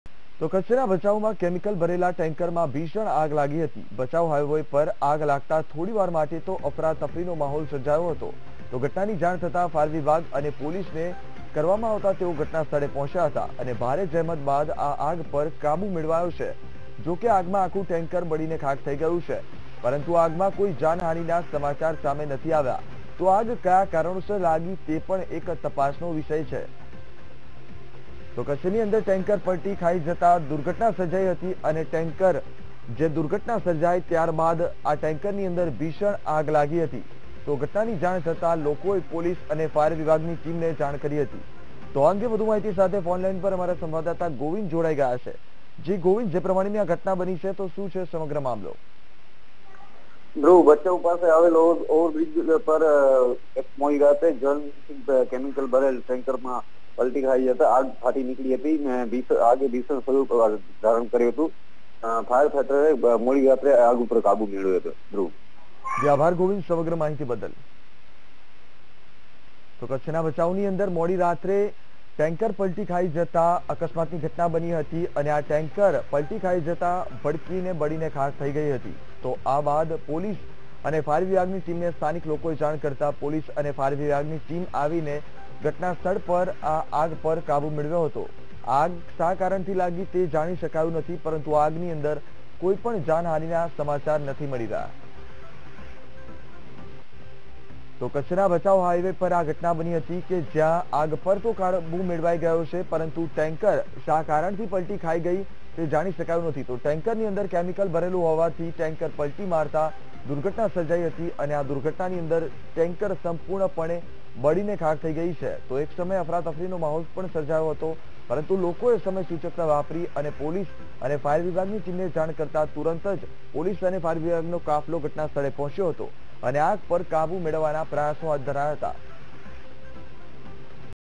Kutch: Chemical ભરેલ Tanker માં 'ભડકો', Bhachau હાઇવે પર સર્જાયા ટ્રાફિક જામના દ્રશ્યો | Vtv
Kutch: Chemical ભરેલ Tanker માં 'ભડકો', Bhachau હાઇવે પર સર્જાયા ટ્રાફિક જામના દ્રશ્યો